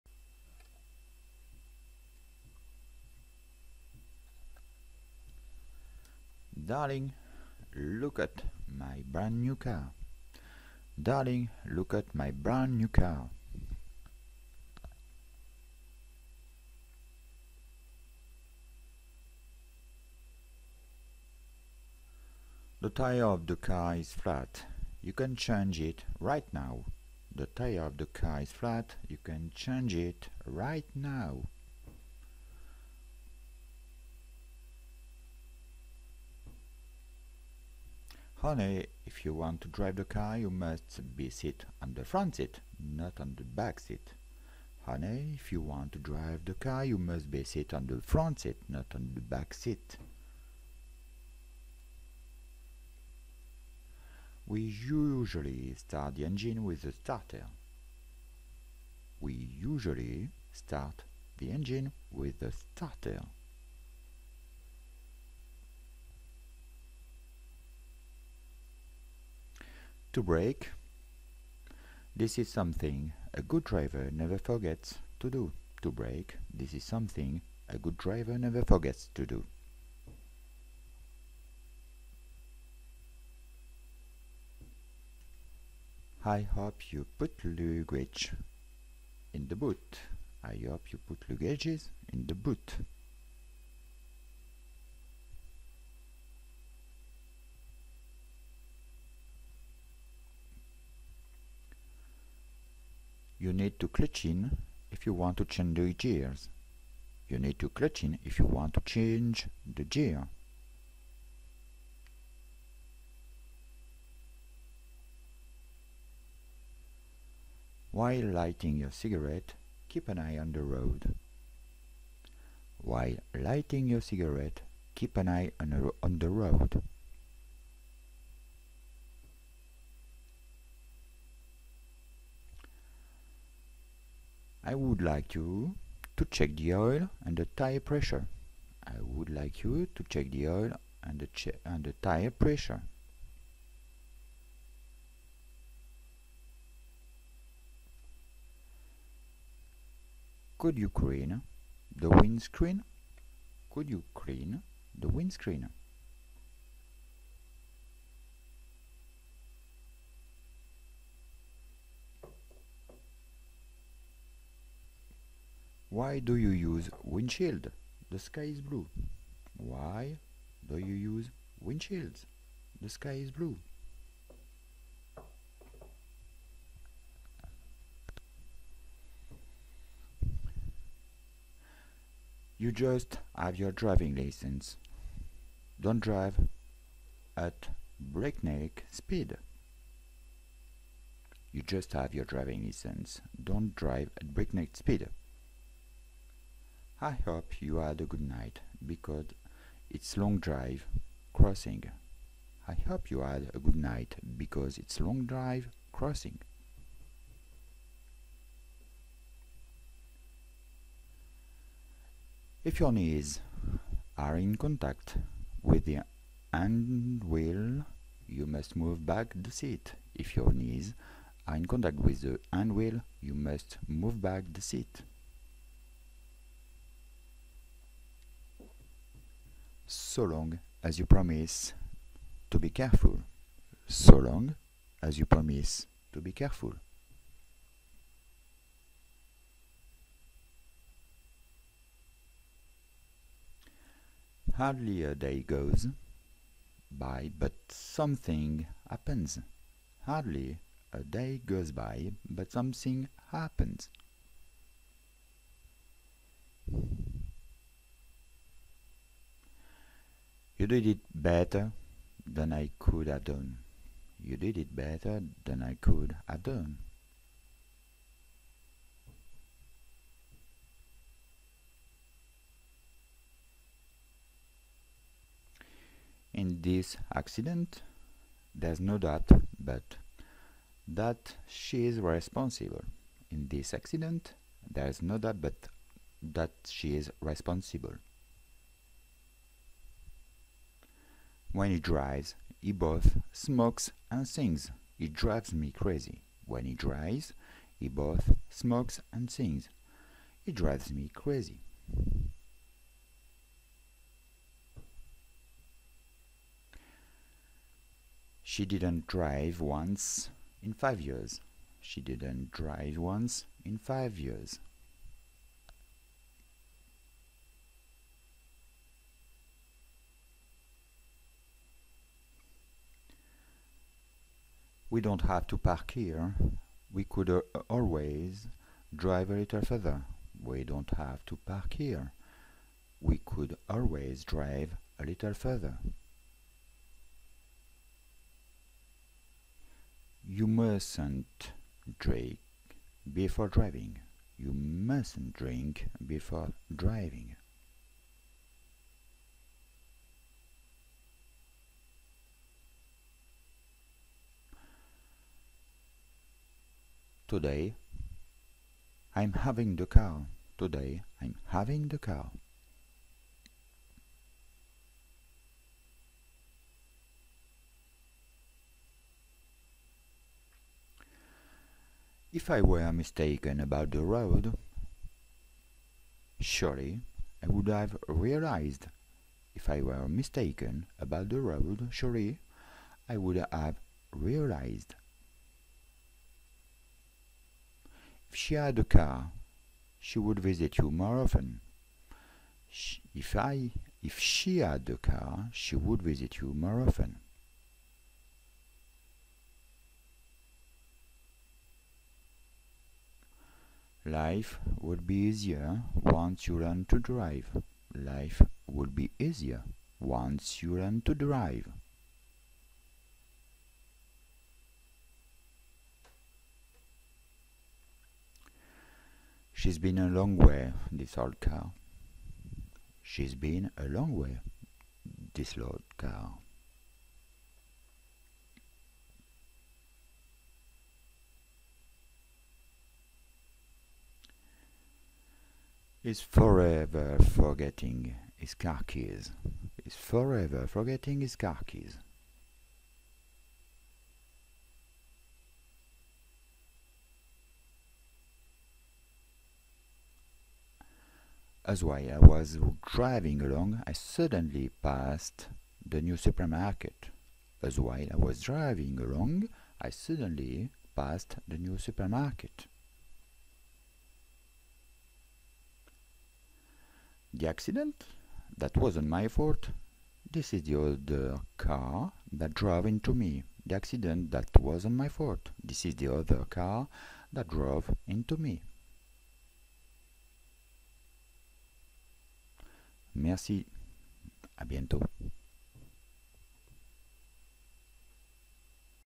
Anglais | En voiture Simone !
Dans cette vidéo, j'ai voulu développer quelques expressions utilisables sur le thème de la voiture : roues, sièges, pneus, etc.

S'il y a des thèmes que vous souhaiteriez voir être abordé, n'hésitez pas à me le faire savoir dans vos commentaire. J'y attacherai une grande attention.

J'espère que cette vidéo vous aura aidé.

N'hésitez pas à la transmettre, à la partager. Et si elle vous a plu, vous pouvez cliquer sur 'j'aime'.